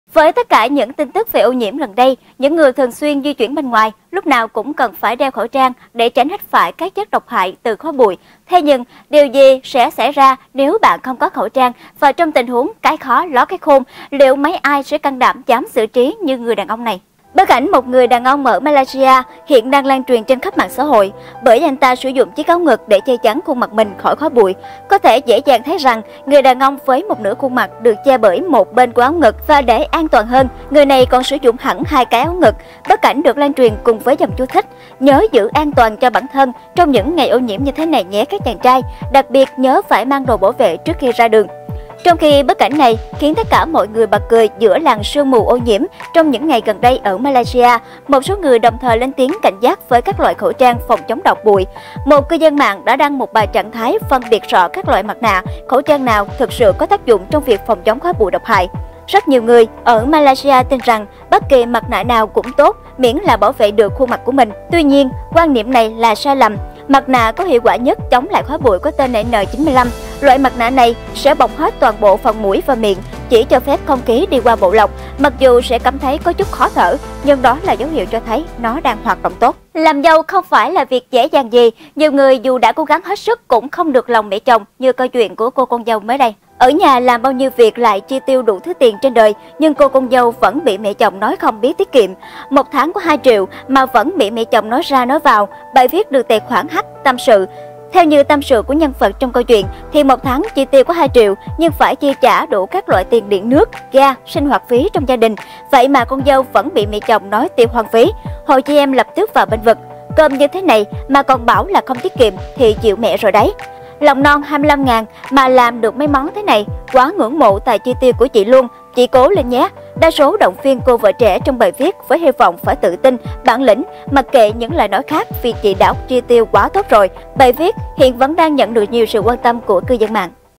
Người đàn ông lấy áo ngực của vợ làm khẩu trang
. Bức ảnh một người đàn ông ở Malaysia hiện đang lan truyền trên khắp MXH bởi, anh ta sử dụng chiếc áo ngực để che chắn khuôn mặt mình khỏi khói bụi. Có thể dễ dàng thấy rằng người đàn ông với một nửa khuôn mặt được che bởi 1 bên của áo ngực. Và để an toàn hơn, người này còn sử dụng hẳn 2 cái áo ngực.